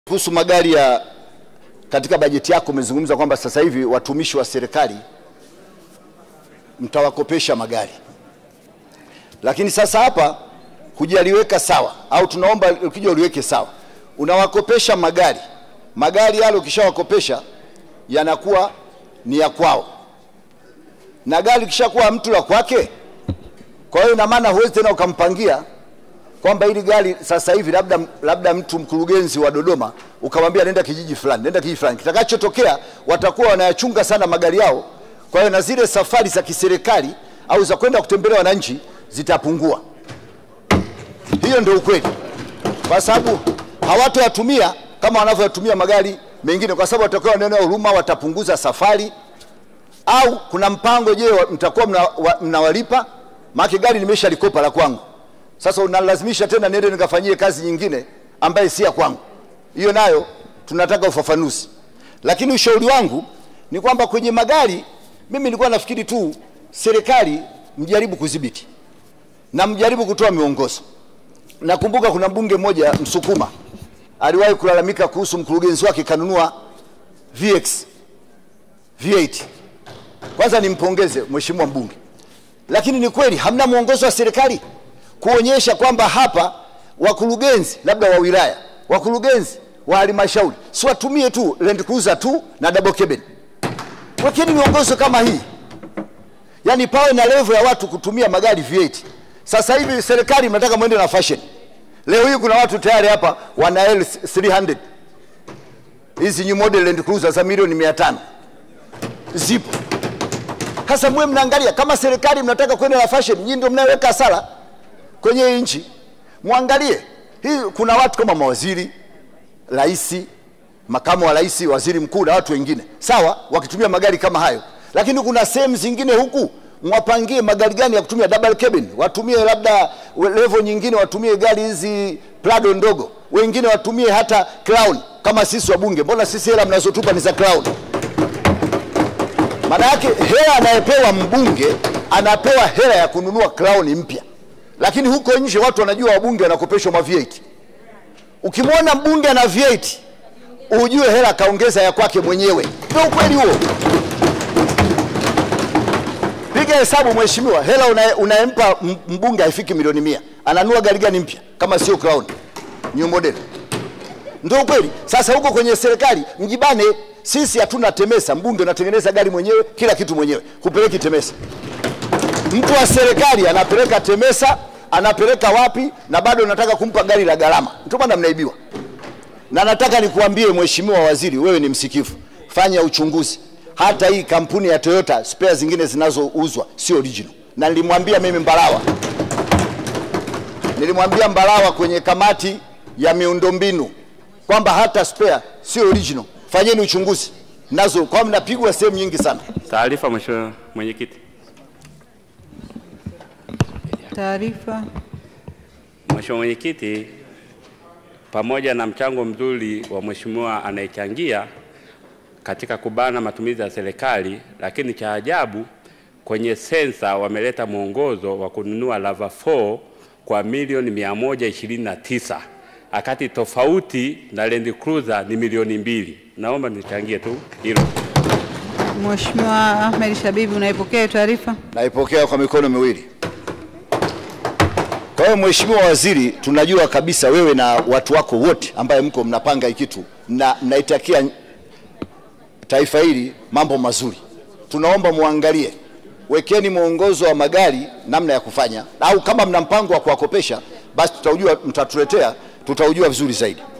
"MAGARI YA WABUNGE, TUNAPEWA HELA YA CROWN" MBUNGE SHABIBY
"Serikali idhibiti kwenye magari na itoe miongozo, ukiona Mbunge ana V8 ujue kaongeza hela ya kwake mwenyewe" - amesema Ahmed Shabiby mbunge wa Gairo, wakati wa mjadala wa Bajeti ya Serikali kwa mwaka wa Fedha 2022/23 Bungeni